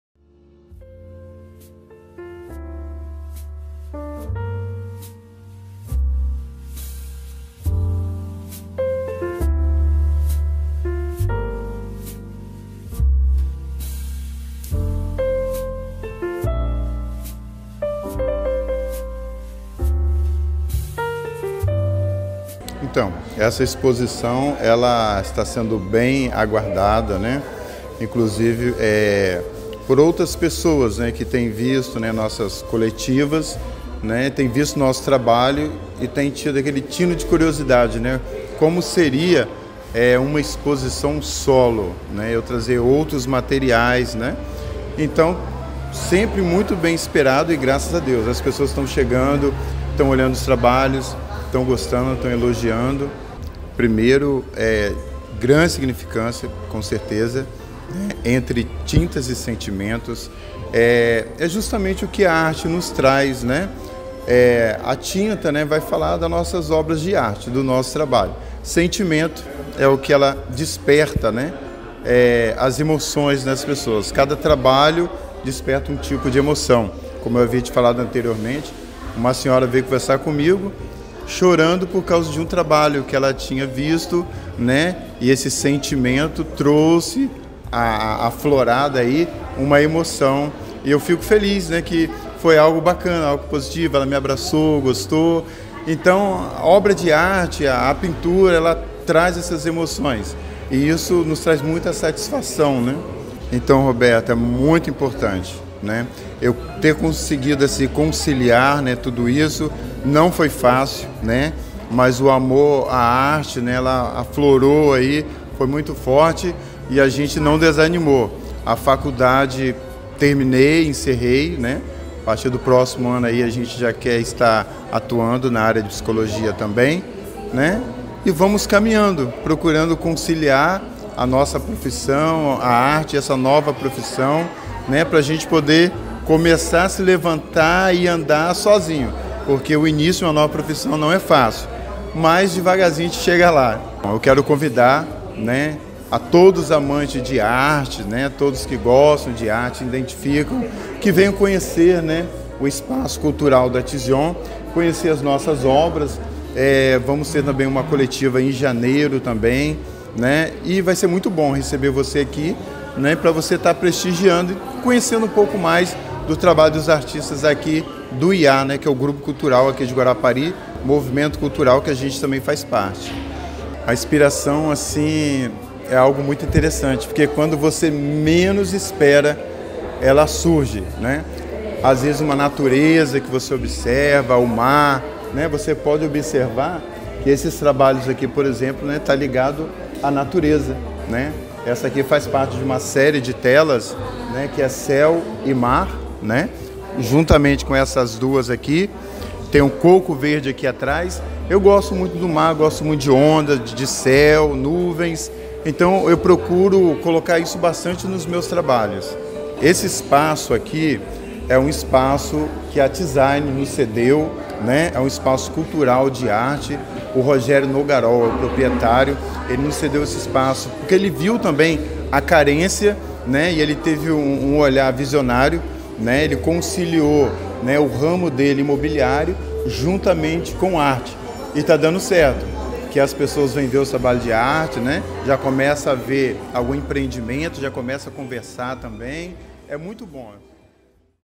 Exposição "Entre Tintas e Sentimentos" é destaque em Guarapari
Aos poucos as artes vão ganhando mais espaço em Guarapari. E as artes plásticas ganharam um espaço de destaque com a abertura da galeria de tarte, Espaço Cultural Tzion, que fica no centro da cidade. É lá que o artista Wanderson Aguiar realizou a sua primeira exposição solo.